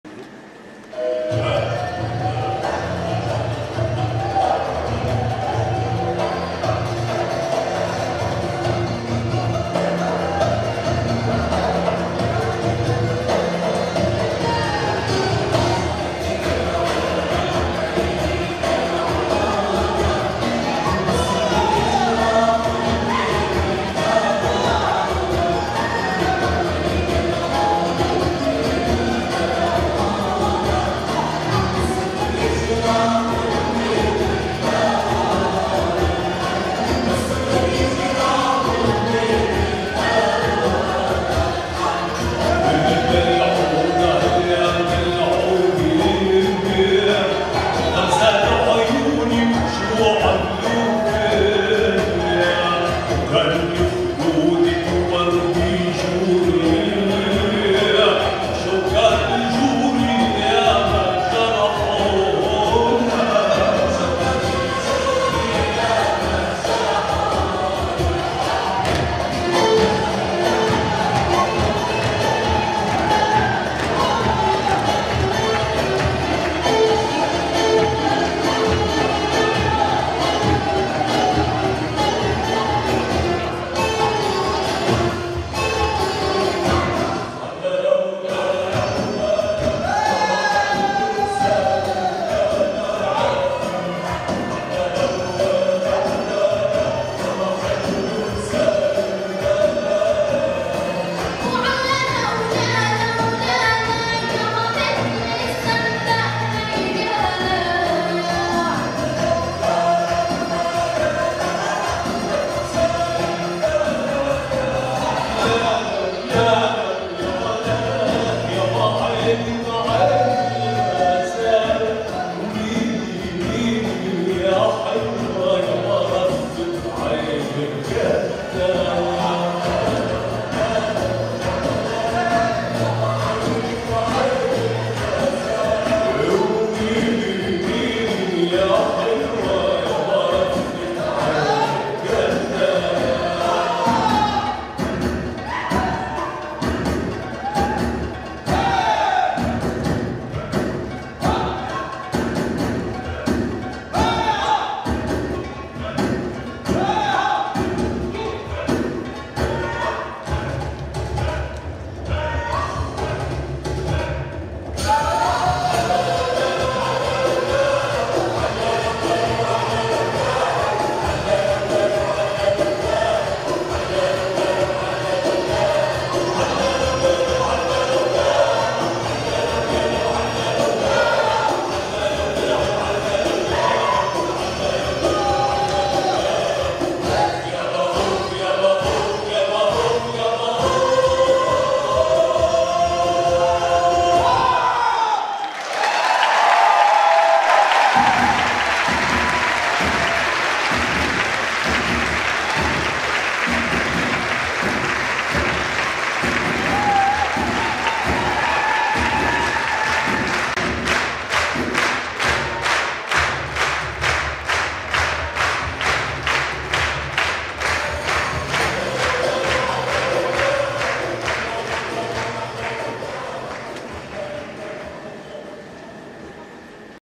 Tala Dance Group : Dabka اجمل دبكة @ Total Oriental 2018
Tala Dance Group from Leipzig/Germany was found in 2017. The men and women from Syria and Germany are dancing arabic folklore dances with modern elements. They share dancing as a common passion to overcome borders.